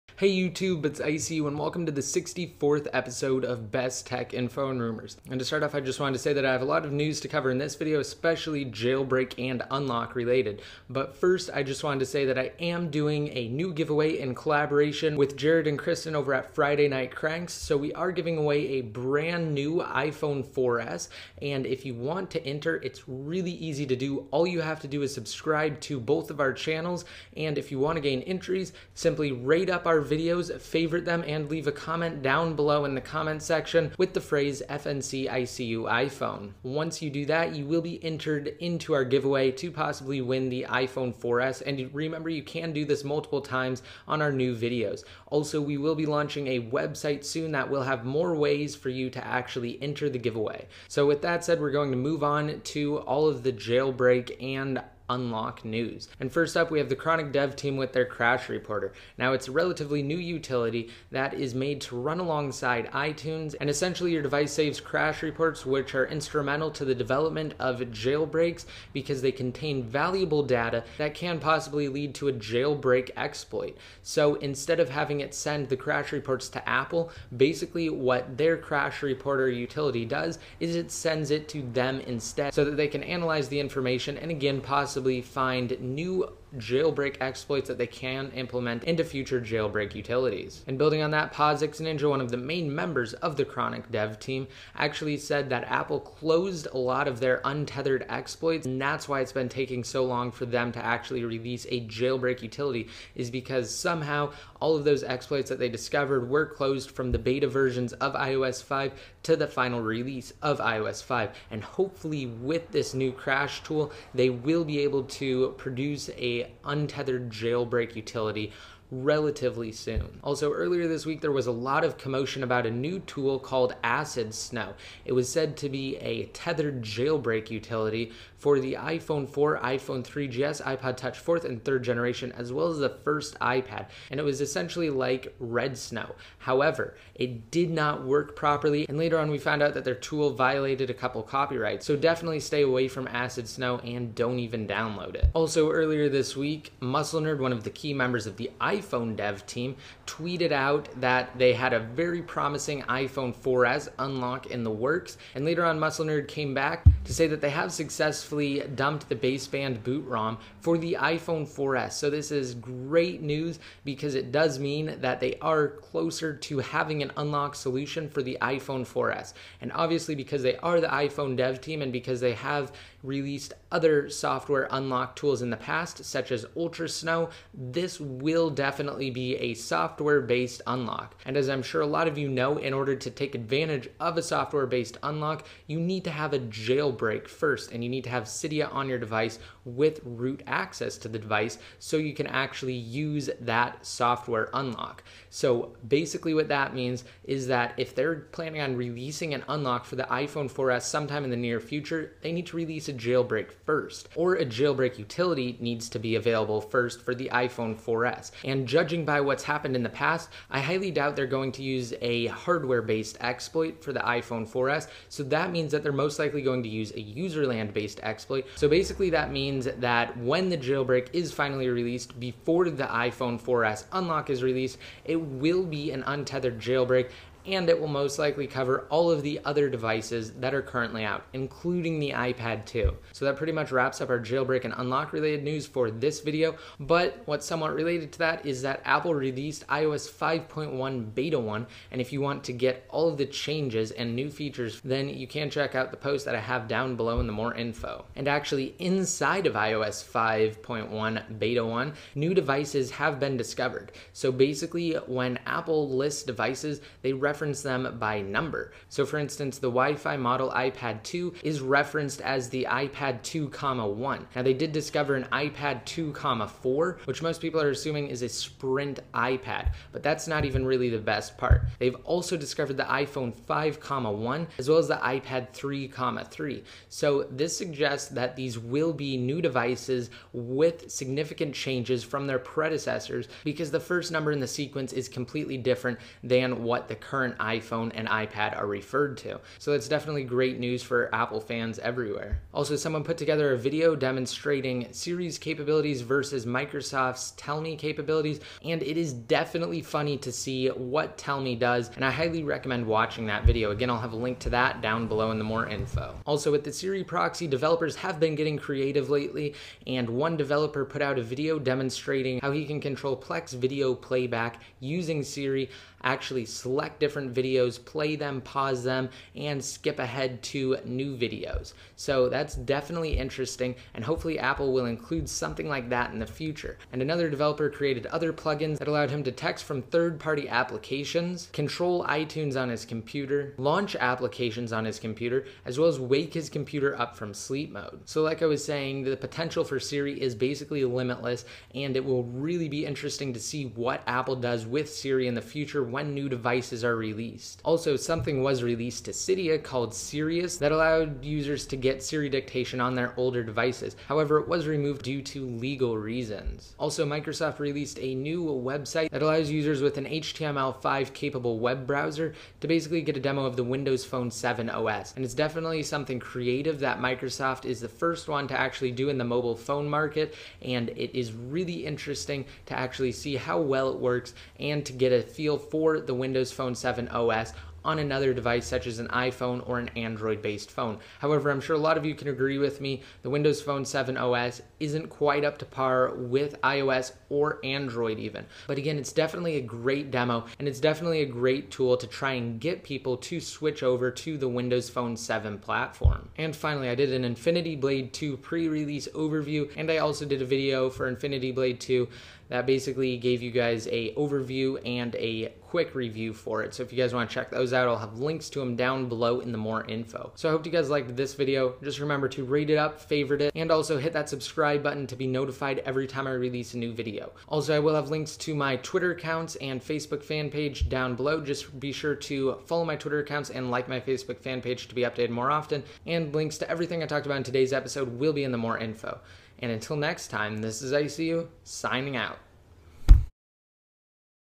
iOS 5.1.1,5.0.1 Untethered Jailbreak Update, iPhone 4S Unlock News, iPhone 4S Giveaway & More
This is the sixty-fourth episode of BestTechInfo And Rumors!

1. iPhone 4S, 4, 3Gs, iPod Touch 4,3 & iPad 2,1 Jailbreak update: The Chronic Dev Team's crash reporter

2. Ac1dSn0w claims to Jailbreak iOS 5 and 5.0.1, bud does it really work?

3. "Very promising" iPhone 4S unlock in the works

7. 5.1 beta 1 features and changes

8. New iPad (iPad 2,4) revealed in the iOS 5.1 firmware

9. Apple's next generation iPhone (iPhone 5,1) and iPad (iPad 3,3) are referenced in iOS 5.1

10. Siri or Tellme, you decide (comparison video)

11. Siri can be used to watch and control videos on plea

12. New Siri Proxy plugins allow for third-party texting, iTunes control, the launching of programs and remote wakeup

13. Siri0us dictation (unfortunately, it was removed from Cydia)

14. How to try the Windows Phone 7 OS on your iPhone or Android phone

15. Infinity Blade 2 overview

16. Infinity Blade 2 review